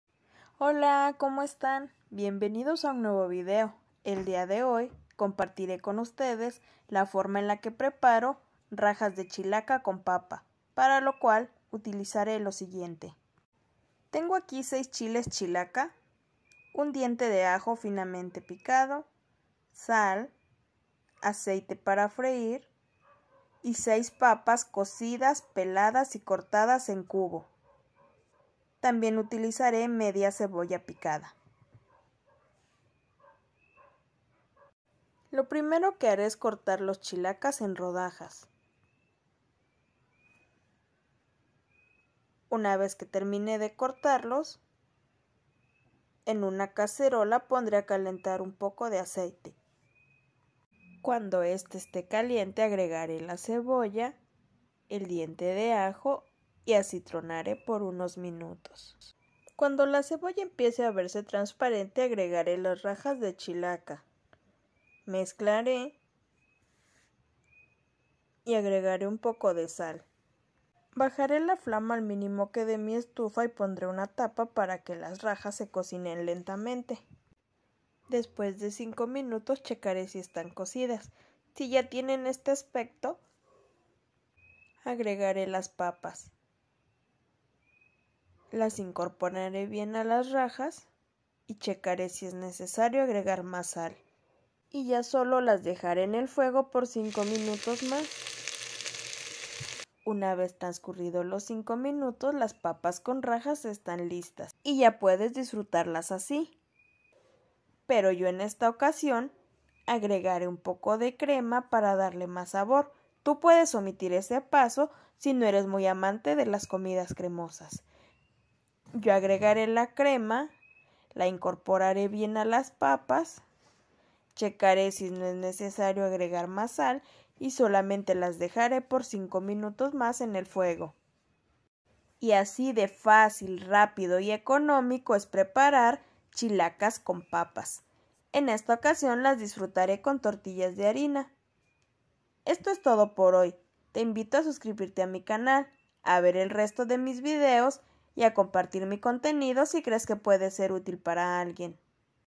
chilacas con papas, receta fácil rápida y económica
cómo preparar unas deliciosas rajas de chile chilaca con papa una receta fácil económica y rápida ingredientes:
6 chiles chilacas

6 papas cocidas peladas y cortadas en cubos

media cebolla picada

un diente de ajo finamente picado

y en caso que así lo prefieras un poquito de crema o media crema